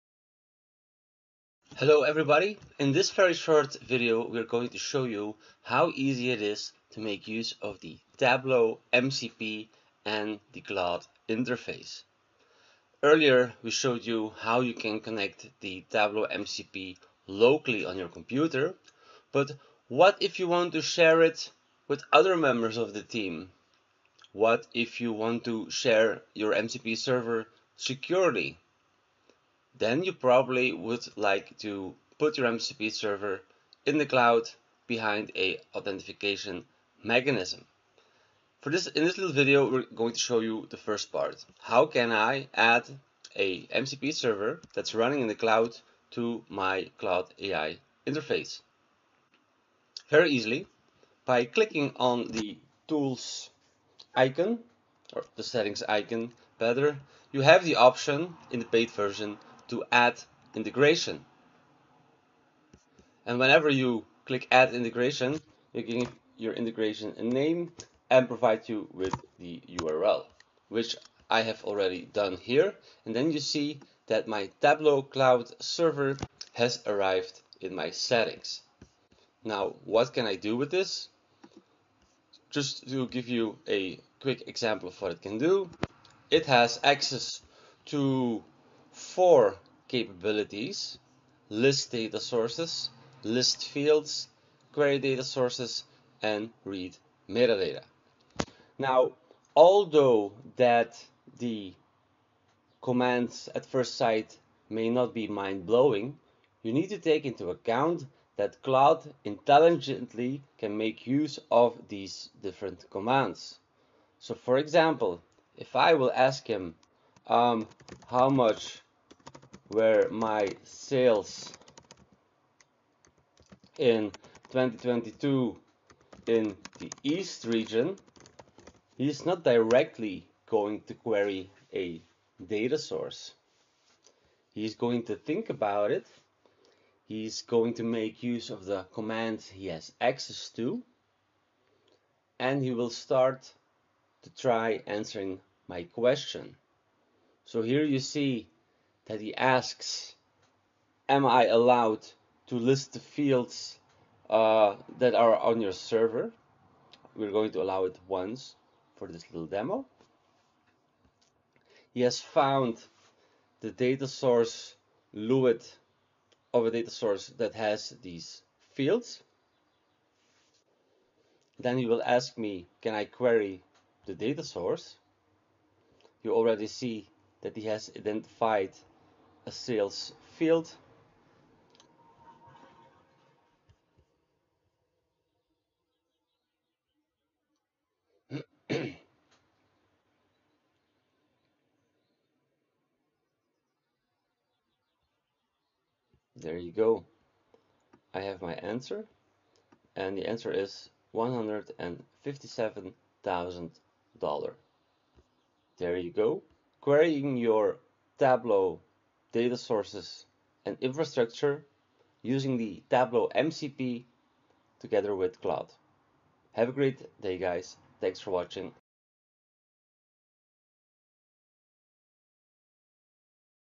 How to Use Tableau MCP + Claude AI - Demo
Explore Data with Ease: Tableau MCP + Claude AI Demo | Conversational Analytics Made Simple

Ready to revolutionize how you explore data? In this short but powerful demo, we’ll show you how to combine Tableau MCP (Metric Content Platform) with Claude AI to unlock seamless, conversational analytics. Say goodbye to manual dashboard digging and hello to intuitive data insights—just by asking questions in natural language.